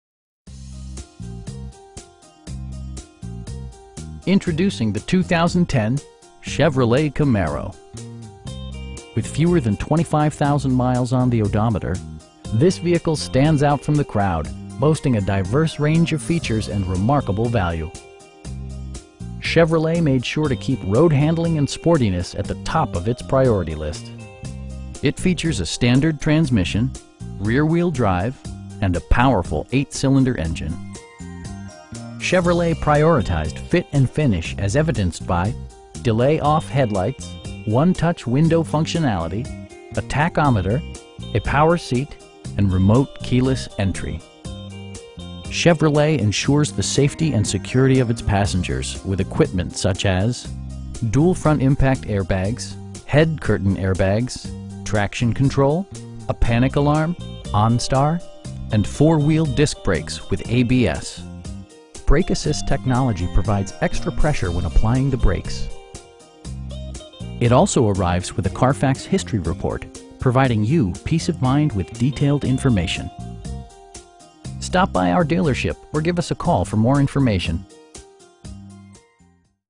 2010 Chevrolet Camaro 1SS in Manassas, VA 20110
Sheehy Nissan - Manassas
9010 Liberia Avenue
in Manassas, VA 20110

Introducing the 2010 Chevrolet Camaro. With fewer than 25,000 miles on the odometer, this vehicle stands out from the crowd, boasting a diverse range of features and remarkable value! Chevrolet made sure to keep road-handling and sportiness at the top of it's priority list. It features a standard transmission, rear-wheel drive, and a powerful 8 cylinder engine. Chevrolet prioritized fit and finish as evidenced by: delay-off headlights, 1-touch window functionality, a tachometer, a power seat, front bucket seats, cruise control, and remote keyless entry. Chevrolet ensures the safety and security of its passengers with equipment such as: dual front impact airbags, head curtain airbags, traction control, a panic alarm, onStar, and 4 wheel disc brakes with ABS. Brake assist technology provides extra pressure when applying the brakes. It also arrives with a Carfax history report, providing you peace of mind with detailed information. Our aim is to provide our customers with the best prices and service at all times. Stop by our dealership or give us a call for more information.